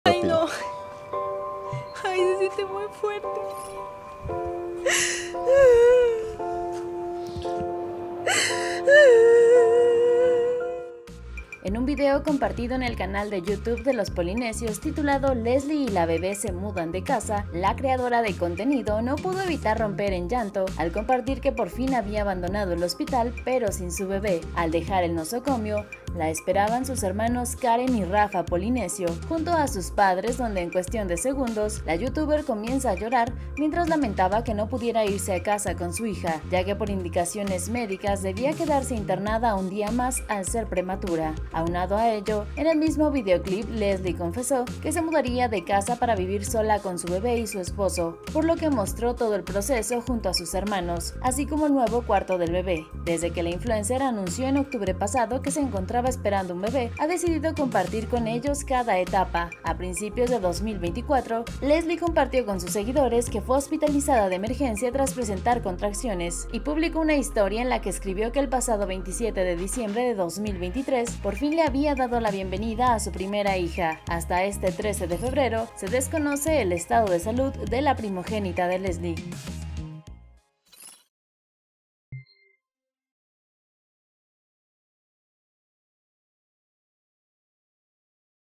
Tras salir del hospital sin su bebé, Lesslie Polinesia rompe en llanto
En un video compartido en el canal de youtube de Los Polinesios, titulado “Lesslie y la bebé se mudan de casa”, la creadora de contenido no pudo evitar romper el llanto